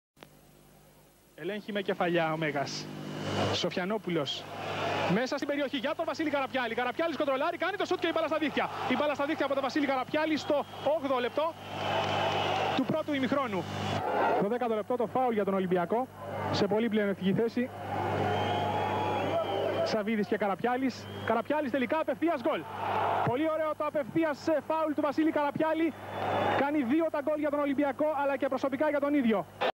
1992-08-16 ΑΙΟΛΙΚΟΣ-ΟΣΦΠ=0-2 ΚΥΠ
ΚΥΡ 16/8/1992 (ΜΥΤΙΛΗΝΗ) ΑΙΟΛΙΚΟΣ - ΟΣΦΠ = 0 - 2 (ΚΥΠ)
[ 8',13'(φ) ΚΑΡΑΠΙΑΛΗΣ.]
ΟΣΦΠ-ΜΙΡΤΣΟΣ ΠΑΧΑΤΟΥΡΙΔΗΣ ΚΑΡΑΤΑΊ΄ΔΗΣ ΜΗΤΣΙΜΠΟΝΑΣ ΚΟΥΣΟΥΛΑΣ ΜΑΥΡΟΜΑΤΗΣ ΤΣΑΛΟΥΧΙΔΗΣ ΣΑΒΒΙΔΗΣ(69'ΤΣΙΑΝΤΑΚΗΣ) ΒΑΊ΄ΤΣΗΣ ΚΑΡΑΠΙΑΛΗΣ(54'ΜΠΑΤΙΣΤΑ) ΣΟΦΙΑΝΟΠΟΥΛΟΣ.
ΠΡΟΠΟΝΗΤΗΣ-ΟΛΕΓΚ ΜΠΛΑΧΙΝ.
ΑΙΟΛΙΚΟΣ-ΚΟΥΤΡΟΥΜΠΙΛΑΣ ΓΑΒΑΛΑΣ ΡΙΠΠΗΣ ΣΑΛΤΑΜΑΡΑΣ ΜΕΓΑΣ ΚΑΣΟΣ ΑΡΜΙΛΑΓΟΣ ΓΙΟΒΑΝΙΔΗΣ(63’ΜΥΡΓΙΑΝΟΣ) ΧΕΛΜΗΣ(46’ΚΑΡΥΩΤΗΣ) ΚΡΟΤΣΙΔΑΣ ΠΕΡΟΒΙΤΣ.
ΠΡΟΠΟΝΗΤΗΣ-ΣΤΑΥΡΟΣ ΣΑΡΑΦΗΣ.
ΔΙΑΙΤΗΤΗΣ-ΧΑΡΛΑΒΑΝΗΣ.
ΚΙΤΡΙΝΕΣ --- ΓΑΒΑΛΑΣ.
ΣΗΜ-ΣΤΟ 43' ΔΕΝ ΔΟΘΗΚΕ ΠΕΝΑΛΤΙ ΥΠΕΡ ΤΟΥ ΟΣΦΠ ΣΕ ΑΠΟΚΡΟΥΣΗ ΜΕ ΤΟ ΧΕΡΙ ΑΜΥΝΤΙΚΟΥ ΤΟΥ ΑΙΟΛΙΚΟΥ ΠΑΝΩ ΣΤΗΝ ΓΡΑΜΜΗ ΤΗΣ ΕΣΤΙΑΣ ΤΟΥ.
ΕΙΣΙΤΗΡΙΑ-6.000